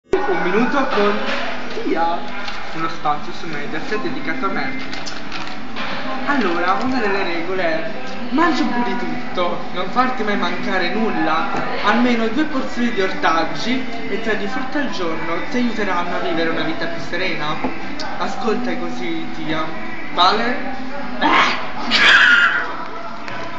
l'angolo di thya @ mcdonalds
thya nella sua rubrica l'angolo di thya da consigli per una alimentazione sana